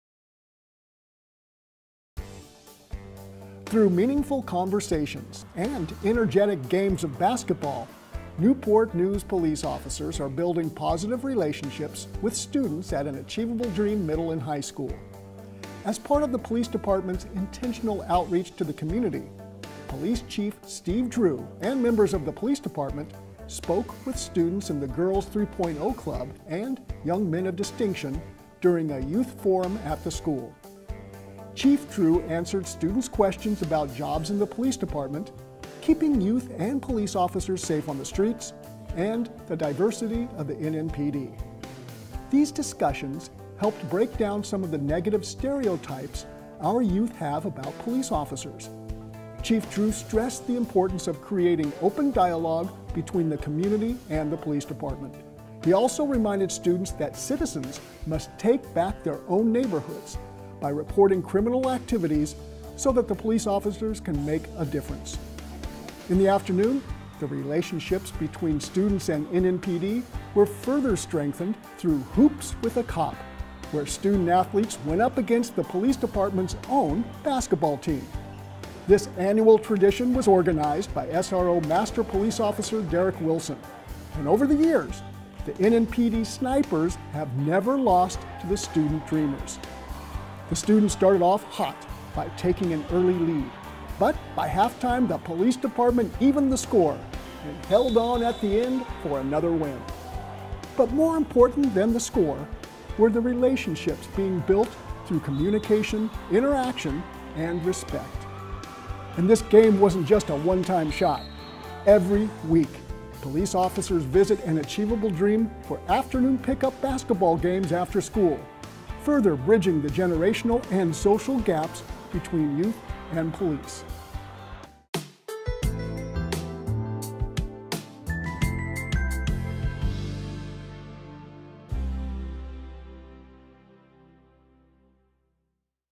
An Achievable Dream Middle High NNPD Forum and Basketball
Thank you to Newport News Public Schools Television for highlighting some of the work we are doing with the youth in Newport News Public Schools.

Over the past several months, we've held youth forums in each of our high schools. During the forums we answer questions from the students and share information about the police department.

Also highlighted in the video, for the past several years we have participated in a Hoops with a Cop event at An Achievable Dream. These events not bring about friendly competition, but they also promote positive interactions between our officers and the community we serve.

VIDEO CREDIT: NNPS-TV